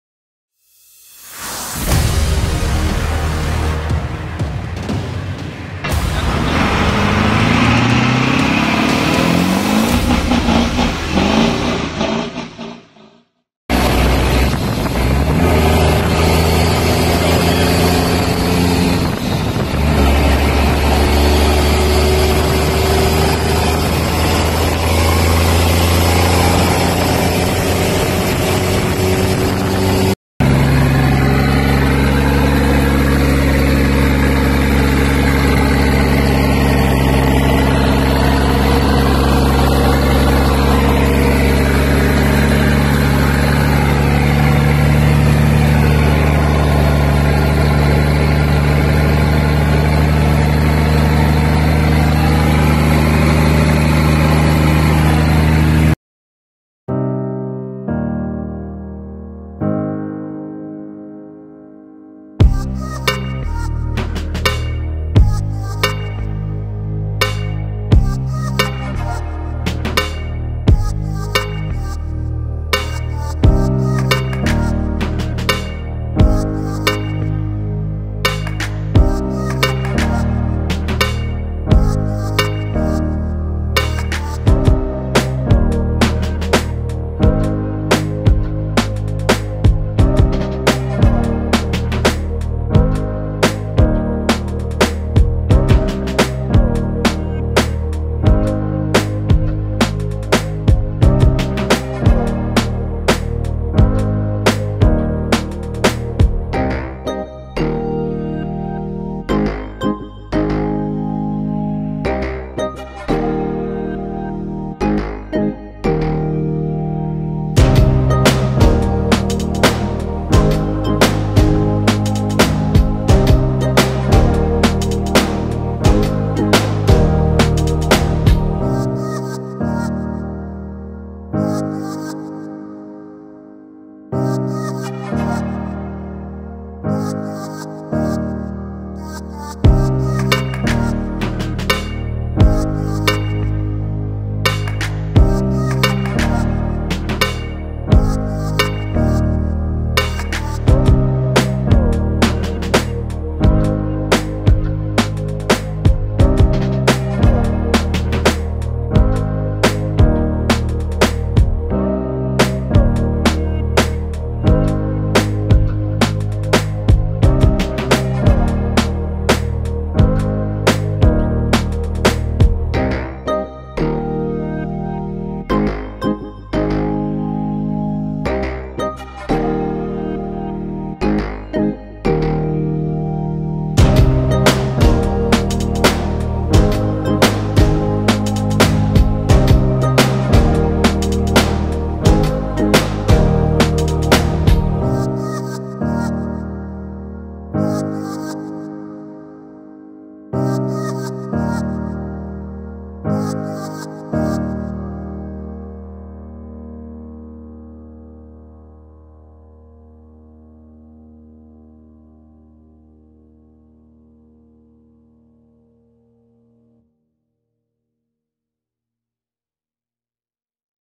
Tropical Depression Imelda Rescue efforts Complete Performance
Sorry guys its fix-it Friday with no Fix-it Friday video. The guys went out to help some friends get out of the floodwaters Thursday down here in Southeast Texas. They took a few videos and pictures to share so here they are